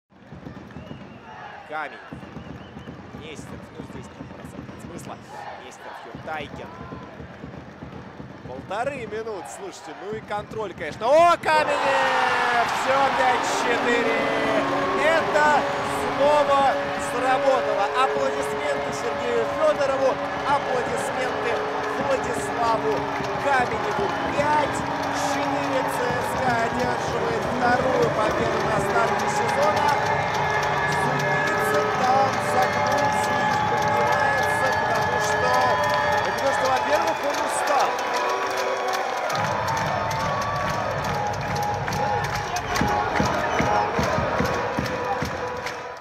Каменев забивает победный гол/Kamenev wins the game for CSKA
Победа за ЦСКА в ОТ благодаря голу Каменева

Kamenev sends it in in OT for CSKA win